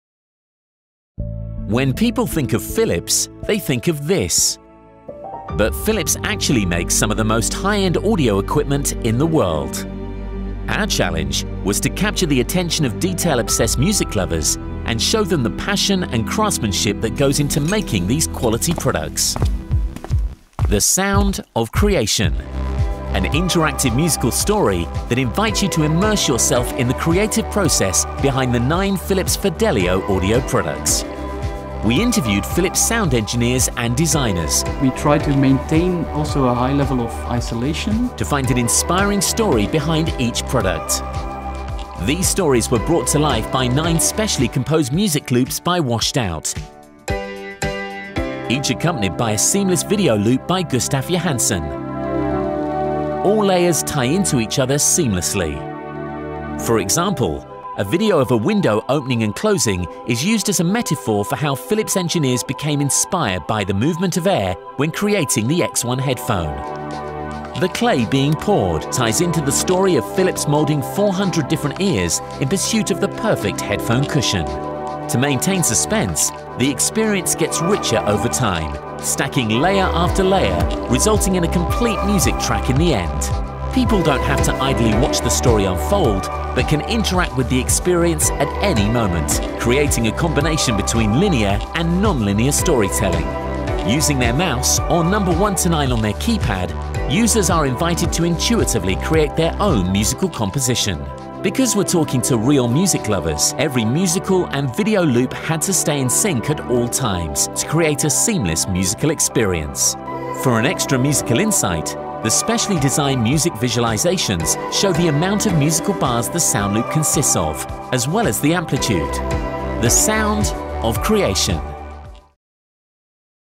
Philips The Sound of Creation
To demonstrate Philips's 'obsession with sound' and show real music lovers the craftsmanship that goes in to creating the brand's audio products. We created an interactive audio-visual story that unfolds over nine chapters; each consisting of an inspirational story, a music loop and a video loop that come together to reveal the true story behind the nine audio products.
The experience gets richer over time, stacking layer after layer, resulting in a complete music track. Users can interact at any moment. By muting chapters they create their own musical composition and find out more about the products.